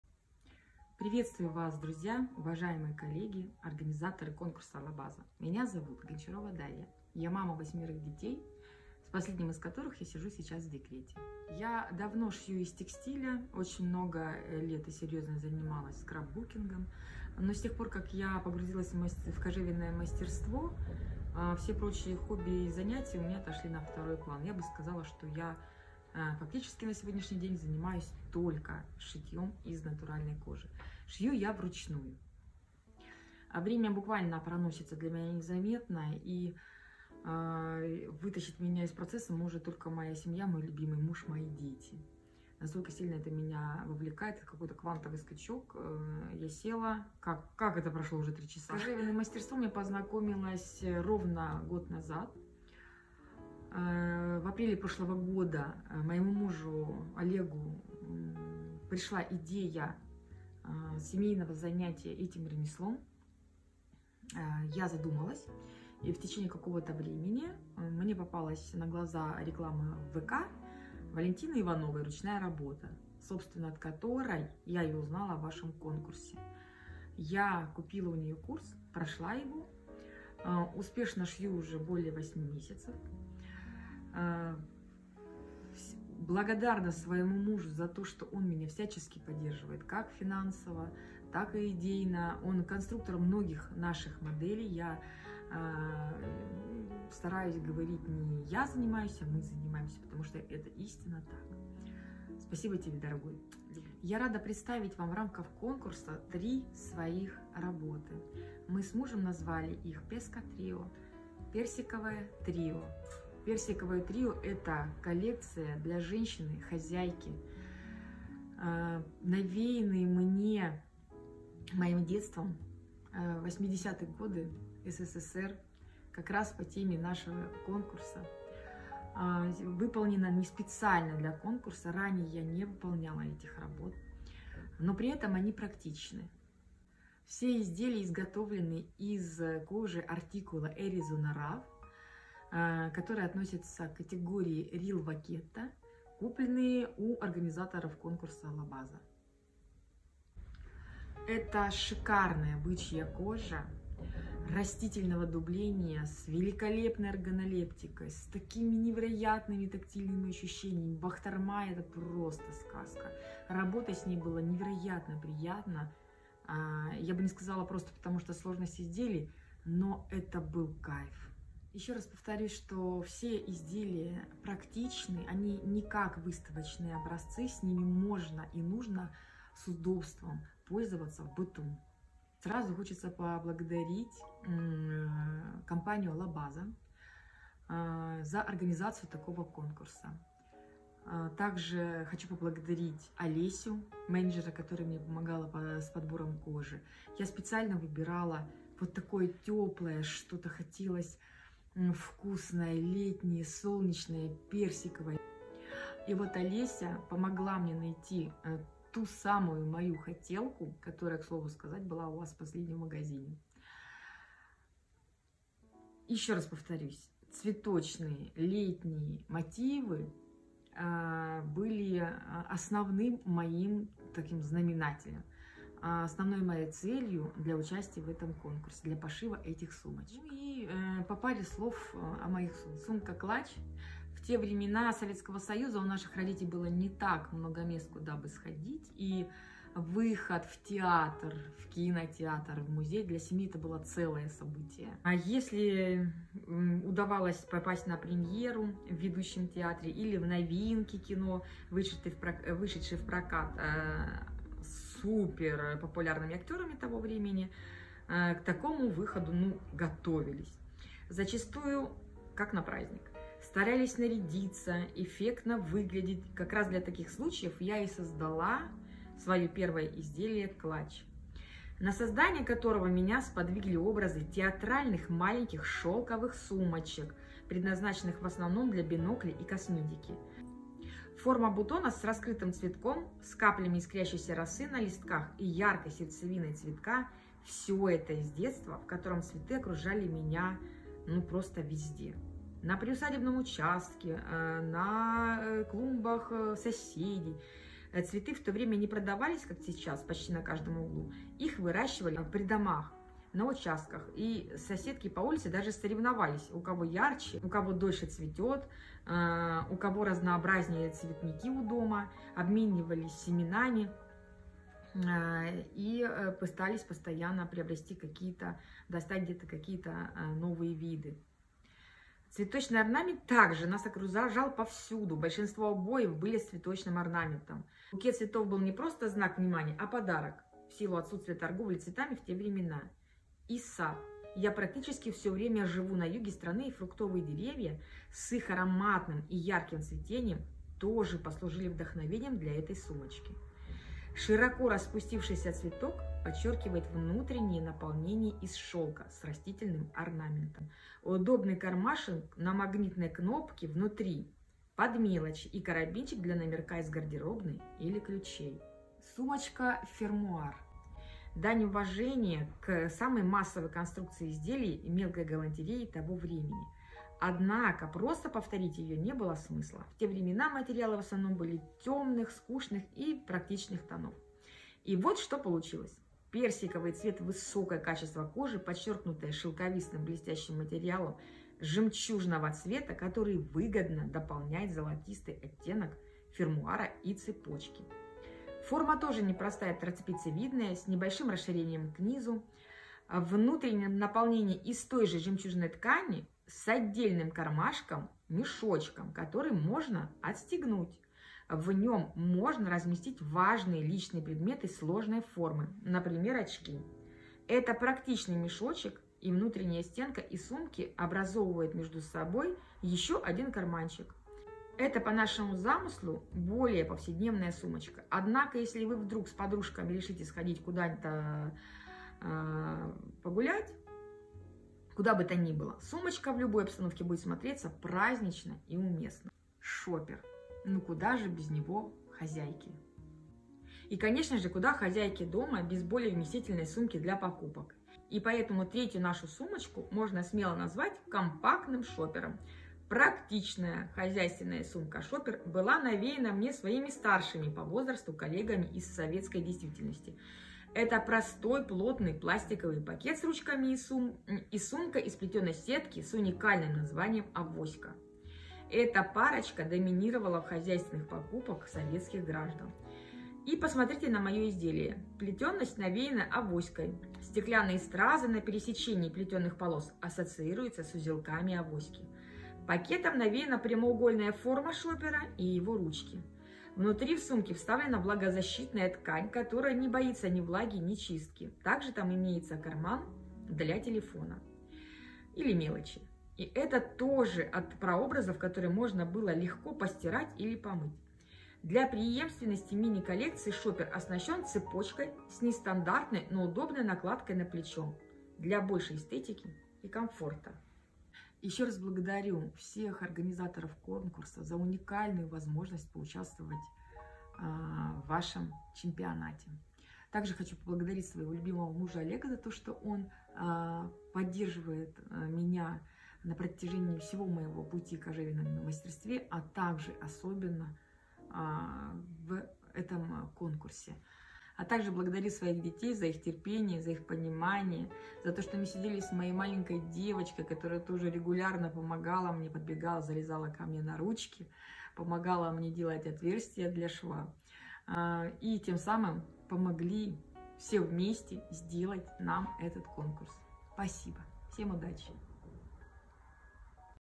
Заняла 4 место на международном конкурсе кожевенников!
Дарья Гончарова - выпускница школы кожевенного ремесла «Ручная Работа». На международном конкурсе кожевенников от магазина итальянской кожи «Albazo» заняла 4 место среди 52 участников!

Своей историей Даша поделилась в видео 😌

Если хотите научиться шить красивые сумки, ремни, ключницы и кошельки из натуральной кожи - приглашаем на бесплатный мини-курс по основам кожевенного дела: